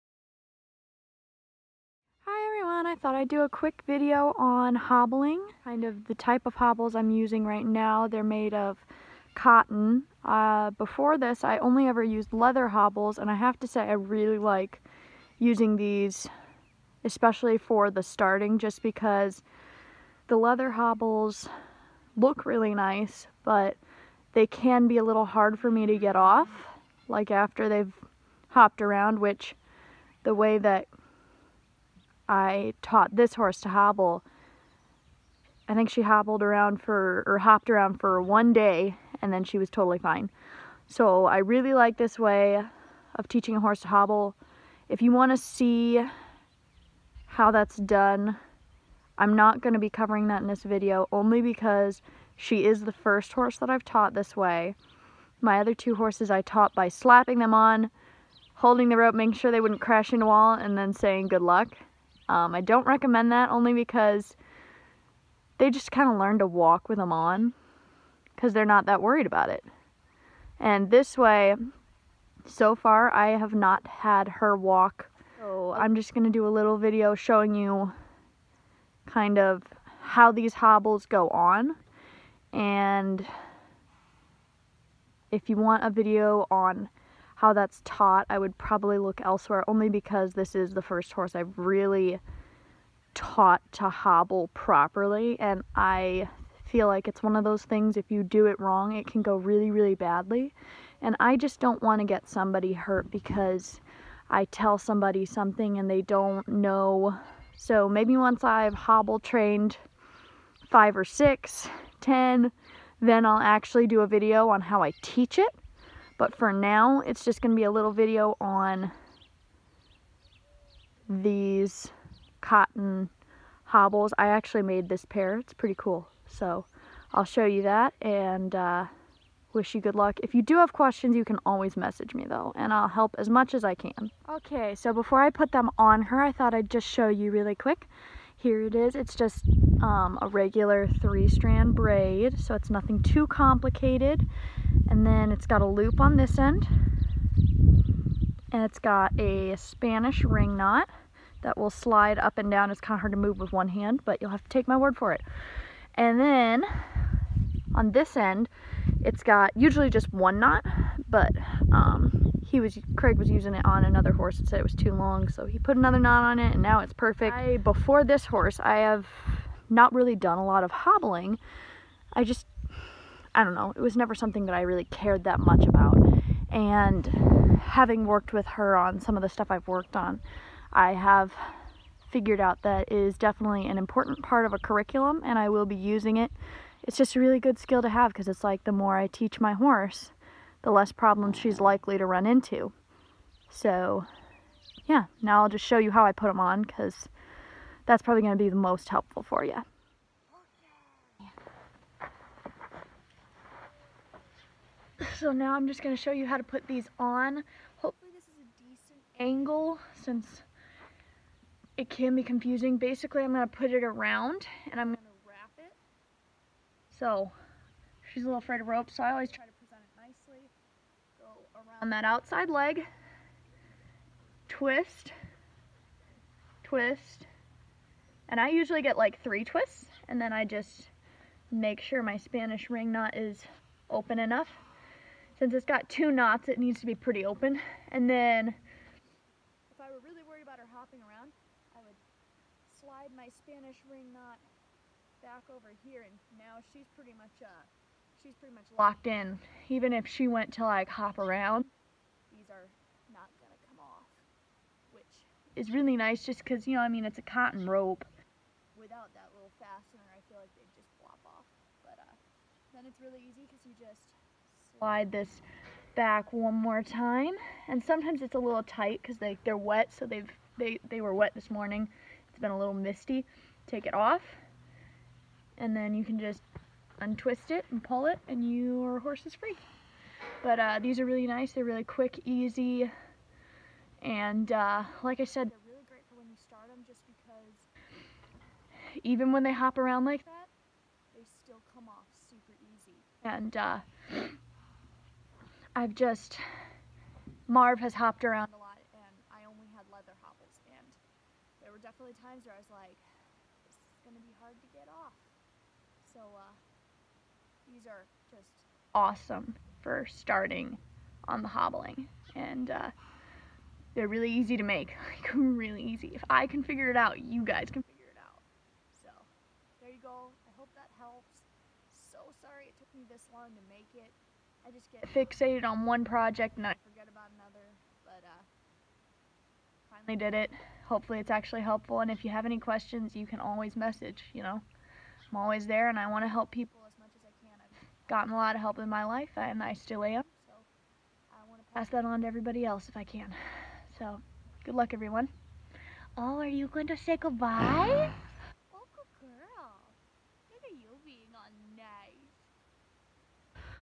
Hobbles for green colts!
This is just a video on the hobbles I use for training horses, hobbles are such an important skill for horses. This is the type of hobbles I use and how to use them. I do not show how I teach it in this video. Please as always ￼￼Exercise caution when working with horses. ￼